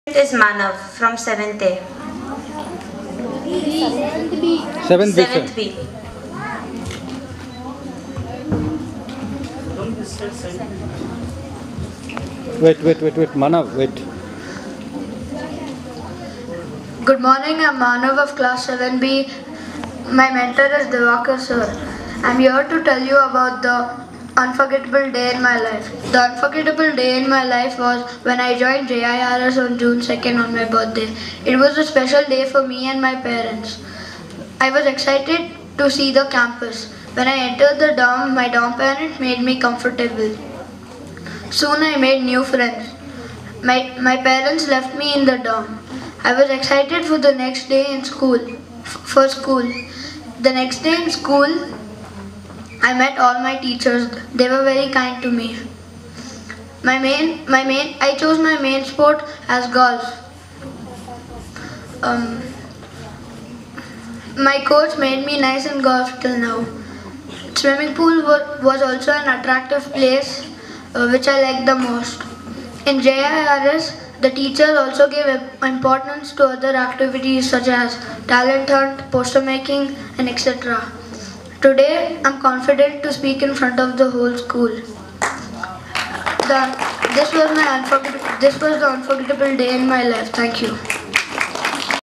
JIRS - LIFE SKILLS DEVELOPMENT PROGRAMME
Life Skills Development Programme - Date: 27 June 18
TOPICS: STORY NARRATION - CBSE class  - 4th, 5th & 6th std
TOPICS: AN UNFORGETTABLE DAY IN MY LIFE - CBSE class 7th
TOPICS: THE IMPACT OF SOCIAL MEDIA ON PRESENT GENERATION  - CBSE - 8TH Students